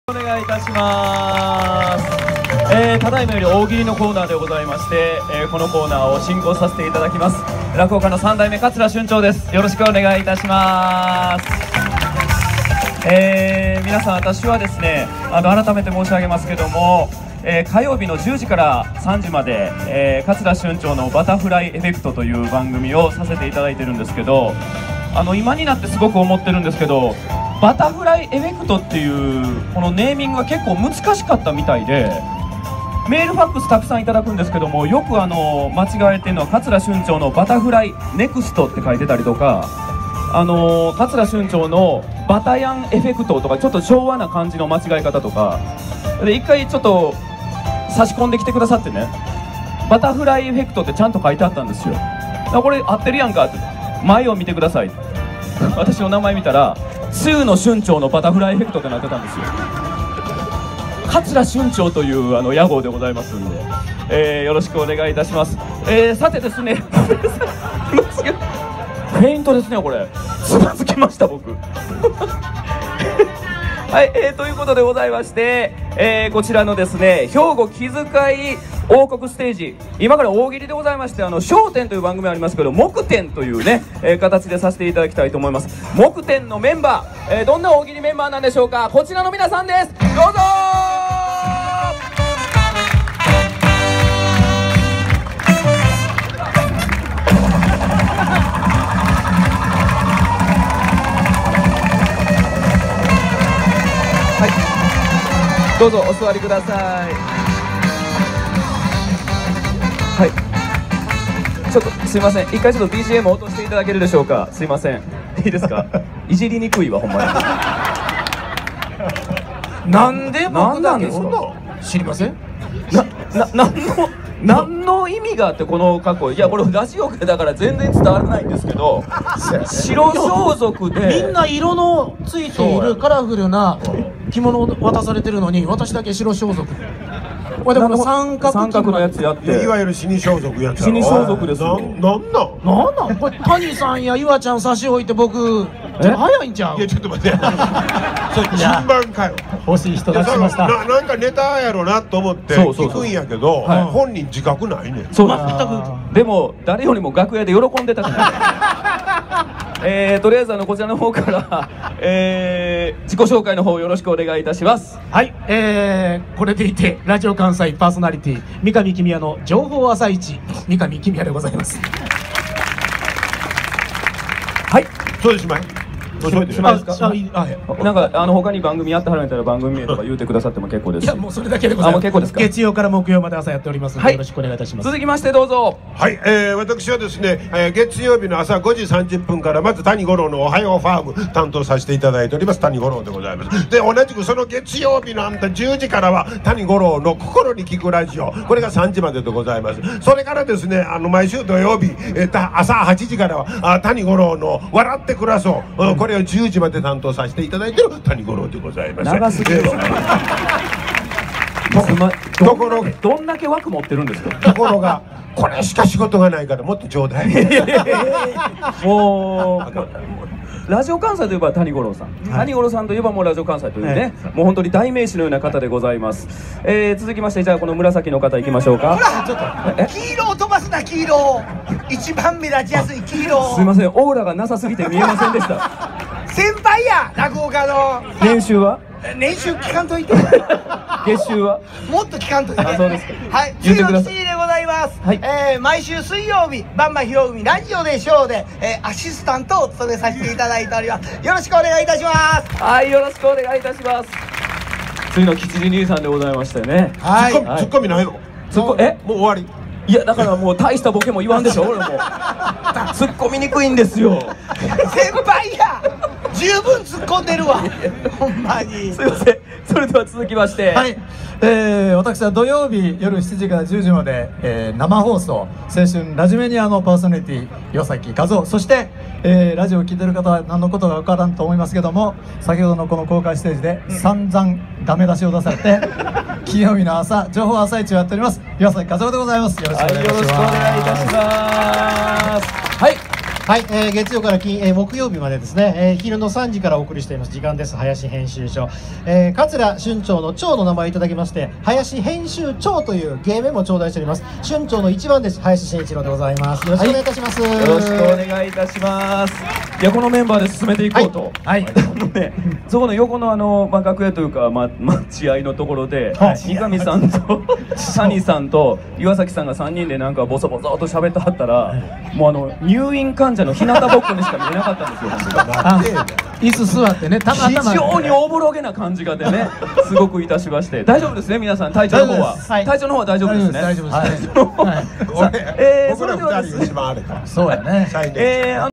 【第20回ラジオ関西まつり】ひょうご木づかい王国劇場『木点』大喜利①
『第20回ラジオ関西まつり～ハーバーボンバー2015』のステージ裏や、出演者からのメッセージなどの映像を配信！

『第20回ラジオ関西まつり～ハーバーボンバー2015』
会場：神戸ハーバーランド高浜岸壁